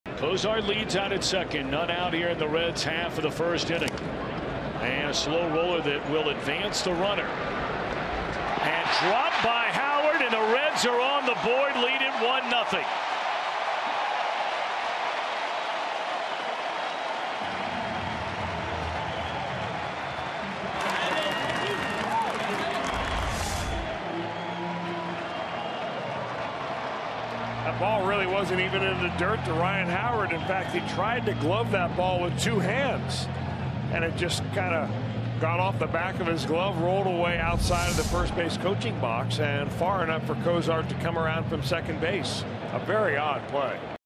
PHI@CIN: Cozart crosses plate on error in the 1st
4/4/16: Zack Cozart comes in to score the game's first run on a missed catch error by Ryan Howard in the 1st inning